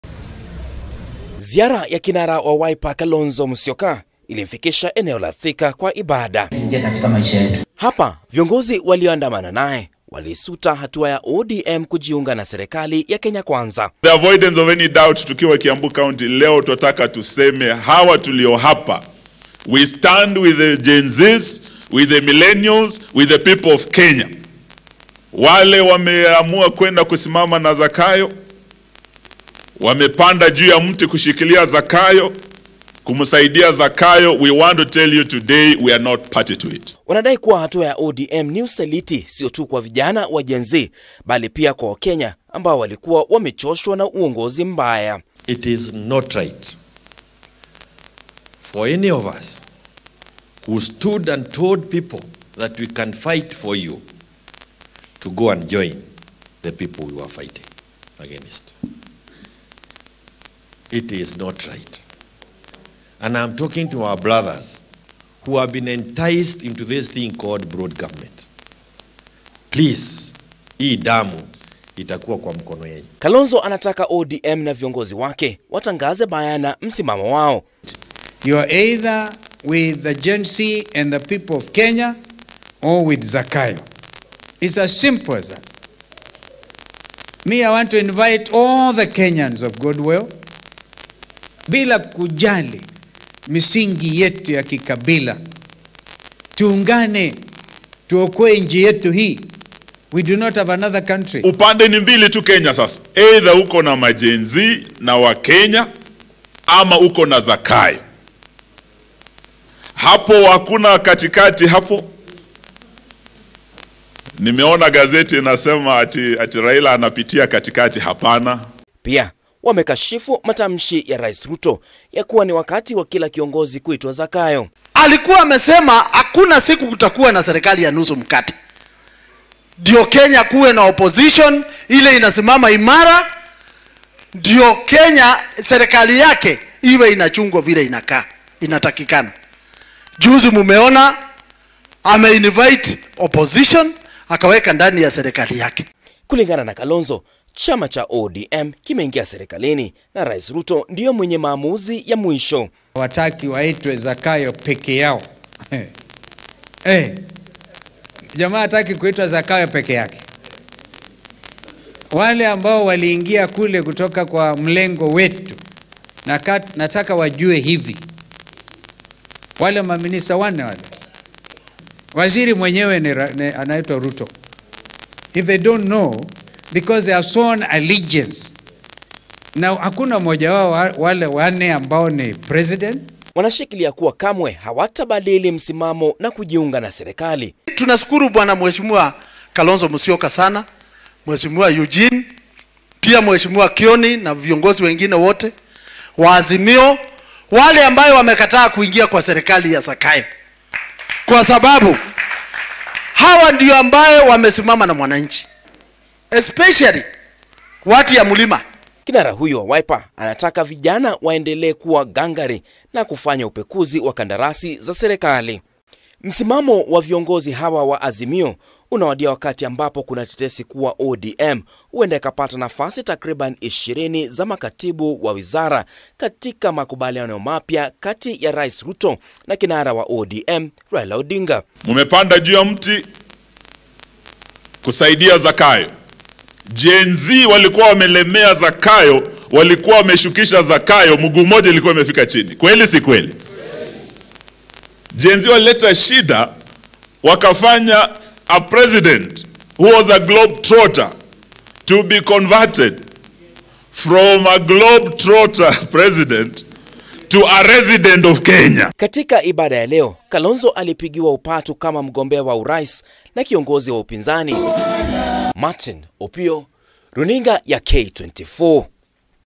Kalonzo achoshwa na kigeugeu cha ODM
Kinara wa Wiper Kalonzo Musyoka ametaka chama cha ODM kijielezeipasavyo kuhusu msimamo wao. Kalonzo aliyefika katika kaunti ya Kiambu kwa ibada amesema kinara Raila Odinga na wenzake wa chama cha ODM watangaze kama  wako pamoja na vijana au serikali kwani  hakuna uwezekano wa kuwa katikati. Kalonzo sasa anataka vijana wa Gen-Z kukagua  kandarasi zinazotolewa na serikali.